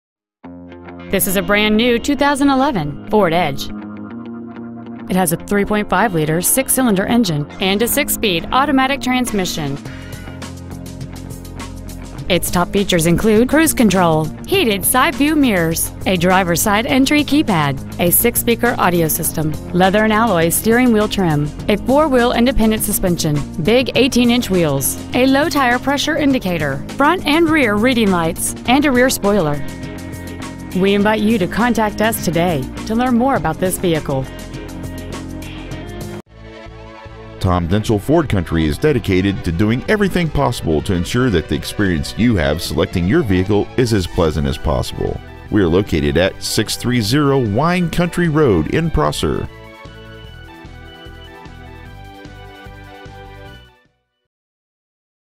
2011 FORD EDGE Prosser WA
We've been honored to serve the Prosser WA area , we promise that your experience at our dealership will exceed your expectations !

 Year : 2011

 Make : FORD

 Model : EDGE

 Engine : 3.5L V6

 Trans . : AUTO 6SPD

 Exterior : KONA BLUE METALLIC

 630 Wine Country Rd